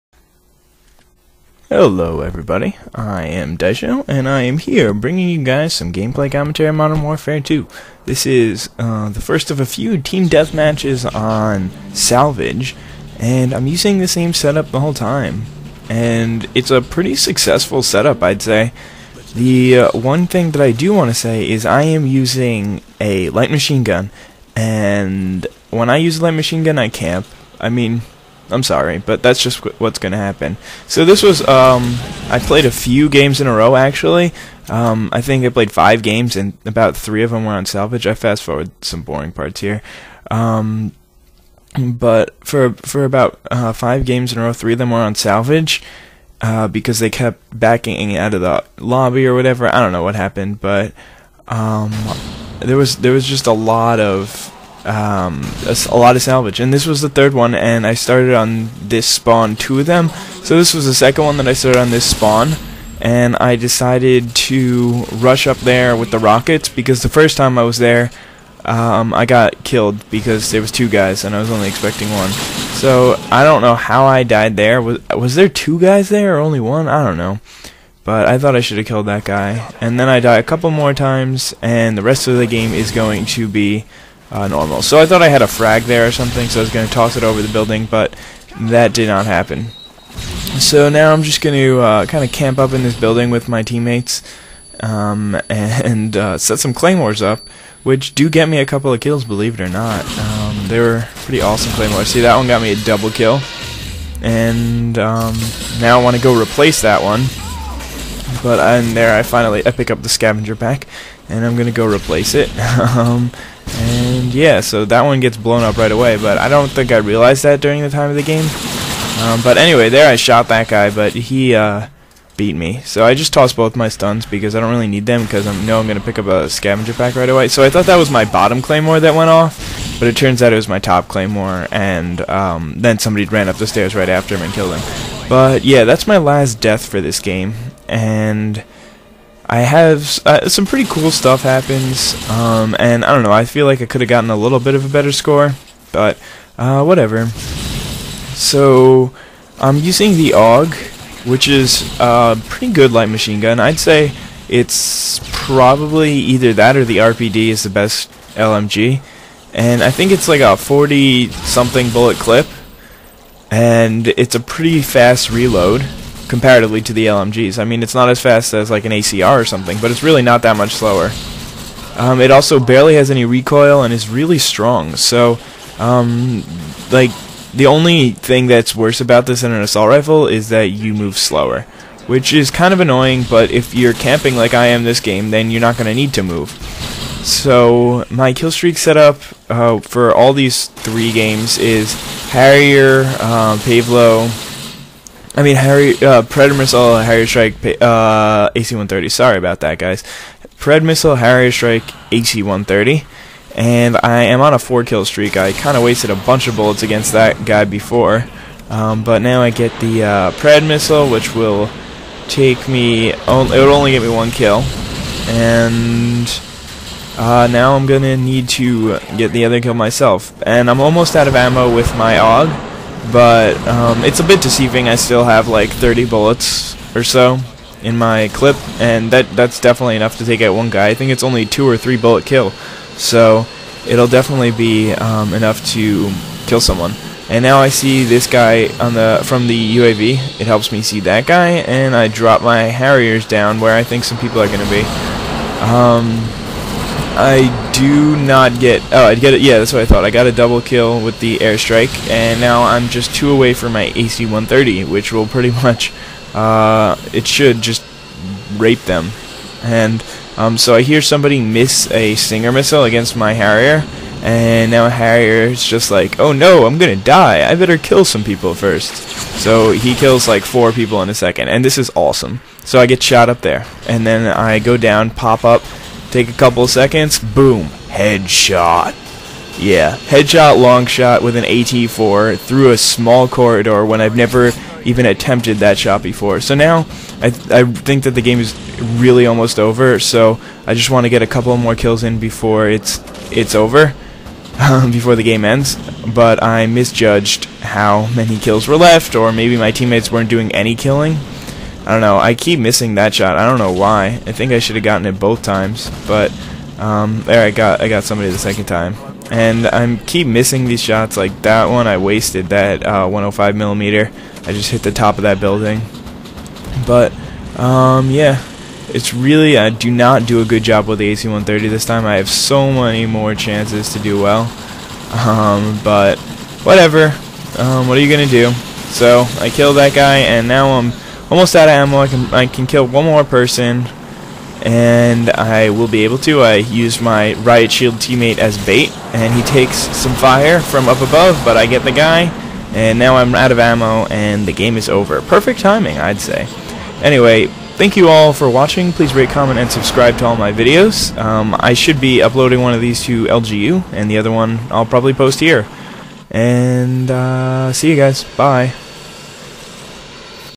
Daisho's Deathmatch: 30-4 on Salvage
Here's a TDM for you guys on salvage.

Setup:
AUG HBAR with grip
AT4
Scavenger
Stopping Power pro
Ninja pro
Claymores
Stuns
Predator Missile
Harrier Strike
AC130